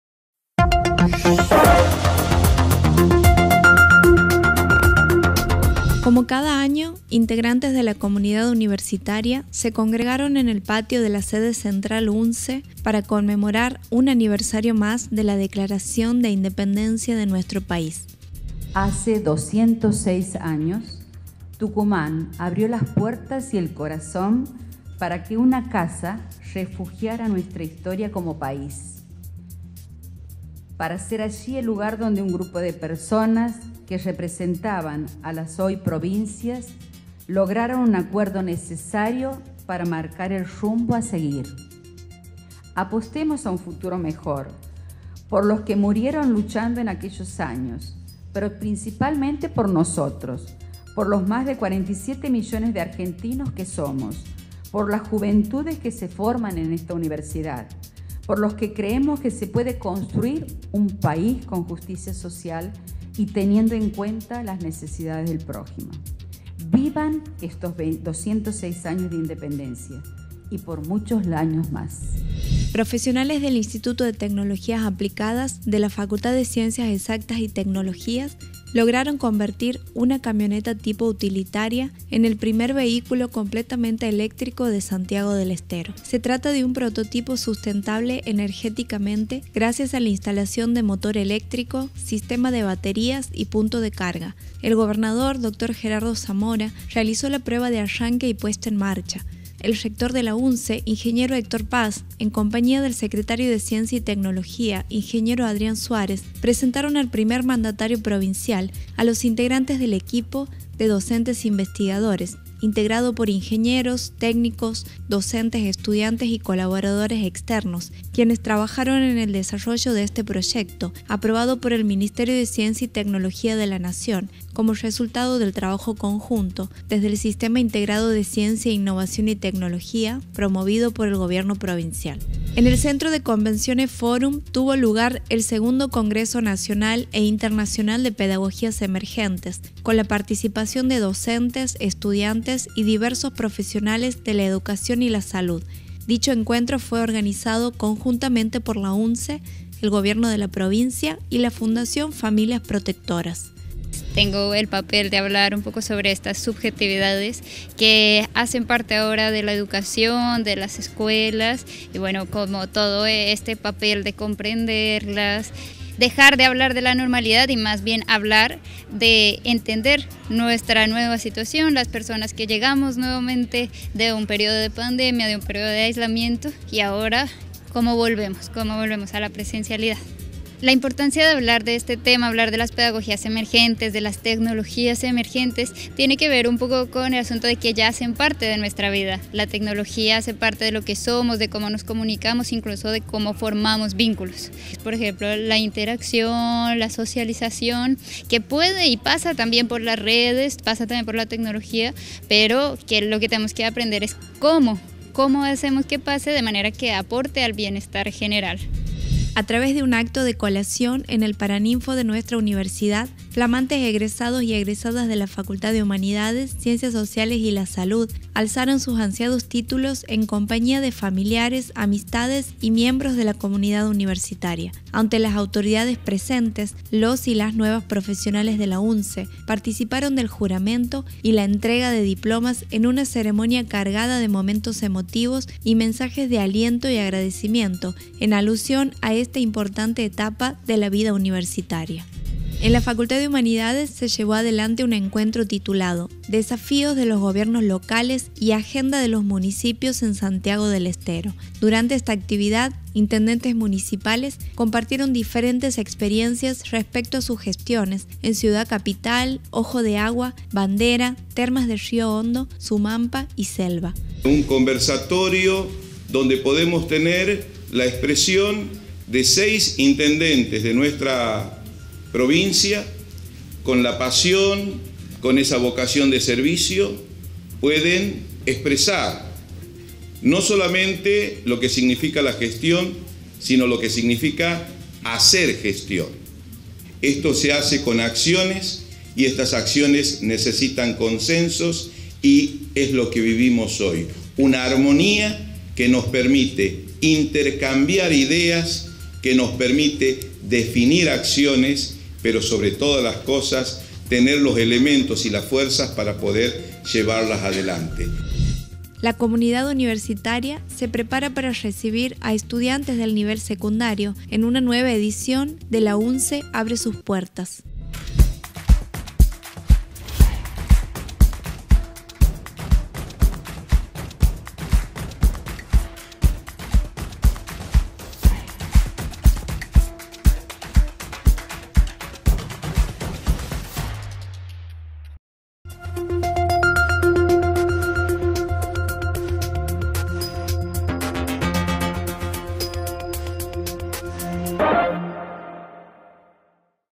INFO UNSE 13/07/2022
Hoy compartimos información sobre:

🔹 206° Aniversario de la Declaración de Independencia.

🔹 Puesta en marcha de la Auto Eléctrico de la UNSE.

🔹 2° Congreso Nacional e Internacional de Pedagogías Emergentes.

🔹 Acto de Colación de la Facultad de Humanidades, Ciencias Sociales y de la Salud.

🔹 Conversatorio “Desafíos de los gobiernos locales y agenda de los municipios en Santiago del Estero”.

🔹 La UNSE Abre sus Puertas 2022.